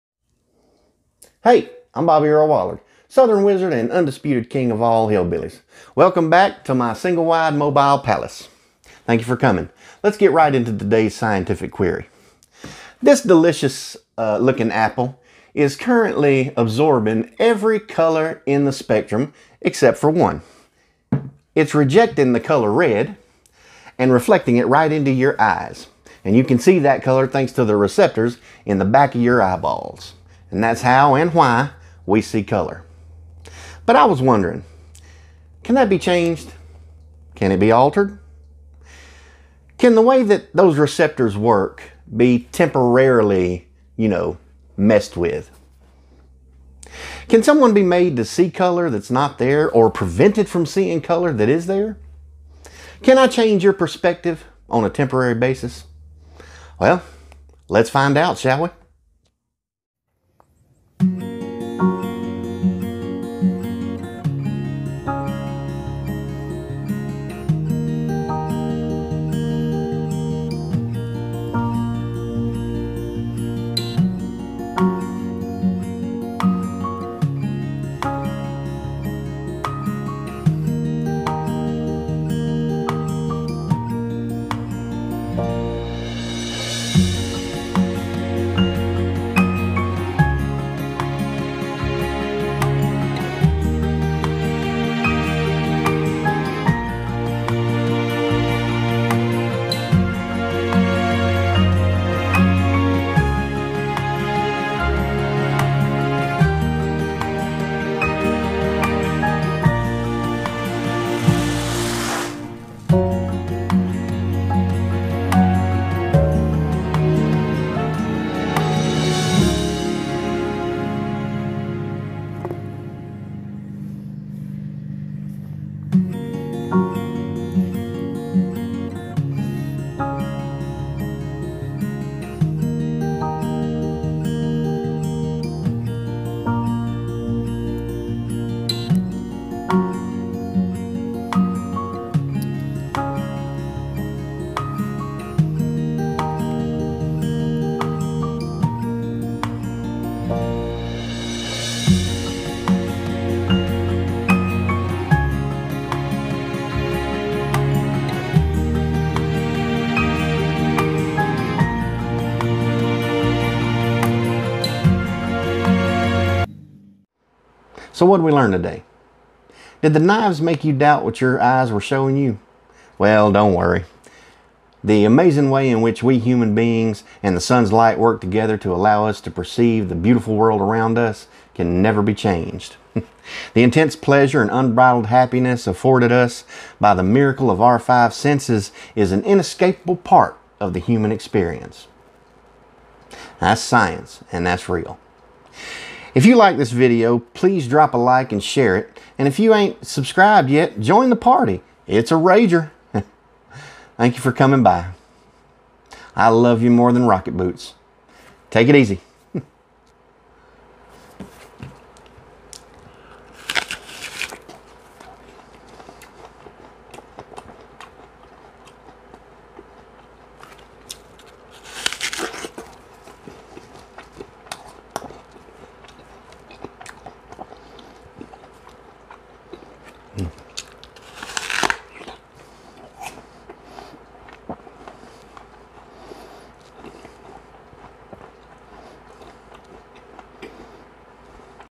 COLOR OF MAGIC
My question today is a simple one with two parts ... what makes us see color, and how can that be altered?